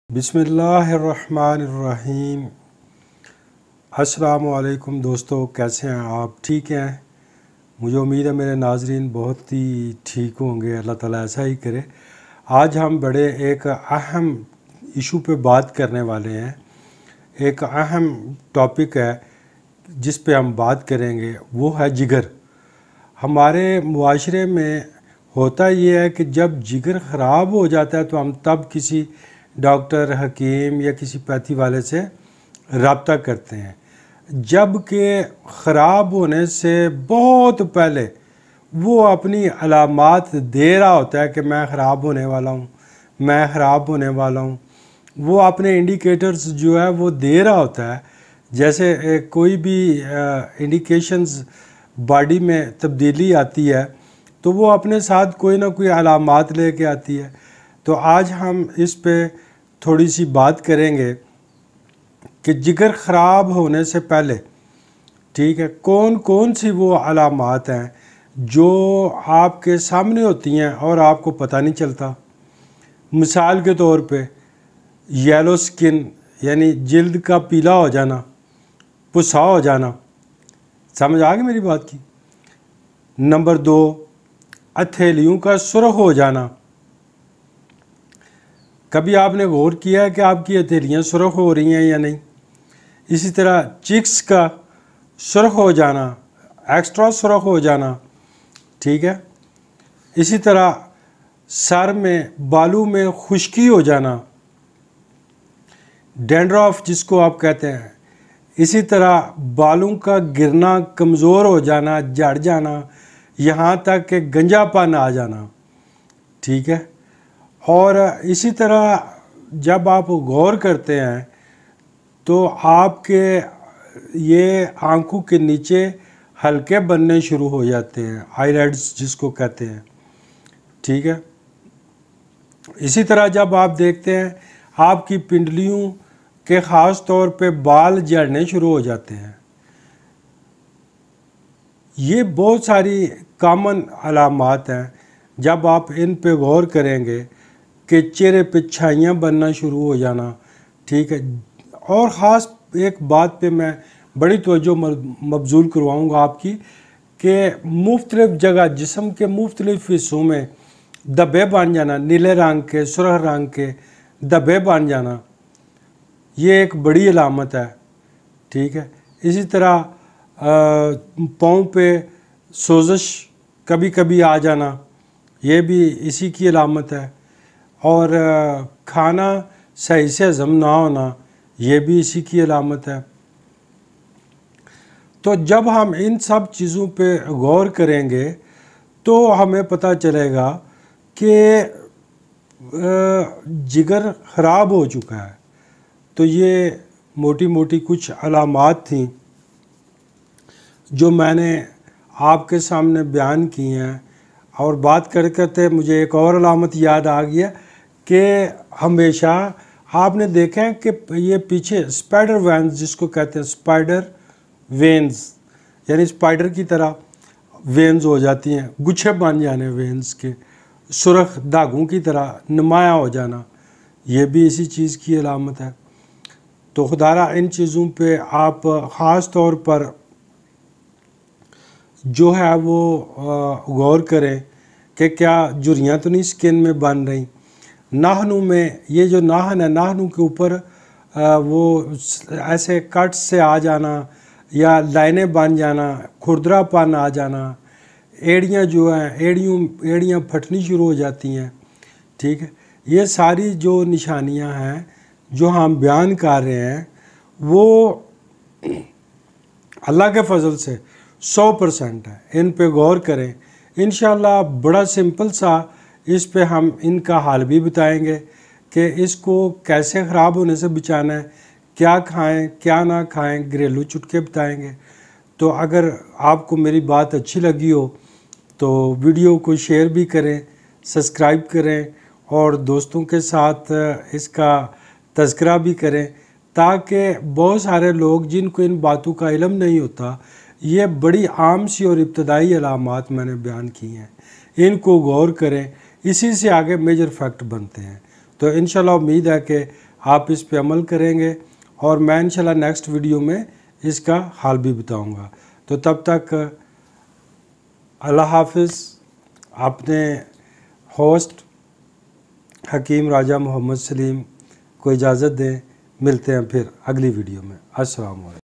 what is the symptom of liver | disorders of liver | common liver problems | Hakeem Raja Saleem
Hello Friends in this video i tell you about the common liver disorder symptoms i hope its much useful and more information for you if you have any question leave a message on WhatsApp or call to me
thanx from
Hakeem TV